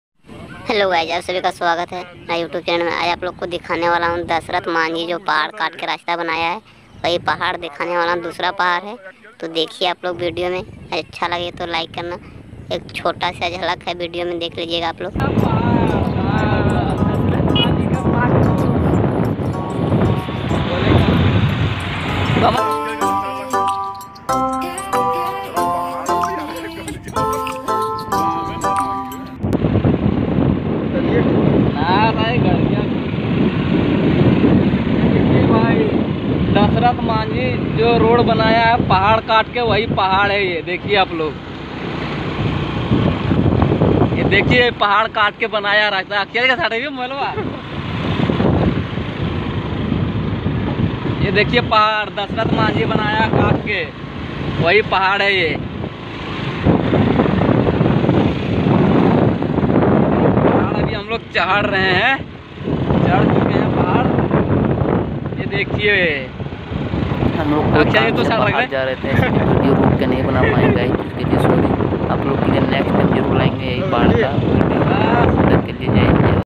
माउंटेन मैन दशरथ माझी की रोड छोटा झलक देखिए Dashrath Manjhi
After 22 years of work, Dashrath shortened travel between the Atri and Wazirganj blocks of Gaya town from 55 km to 15 km. He travelled to New Delhi to get recognition of his work and was rewarded by then Chief Minister of Bihar, Nitish Kumar. In 2016, Indian Post issued a postage stamp featuring him.

videos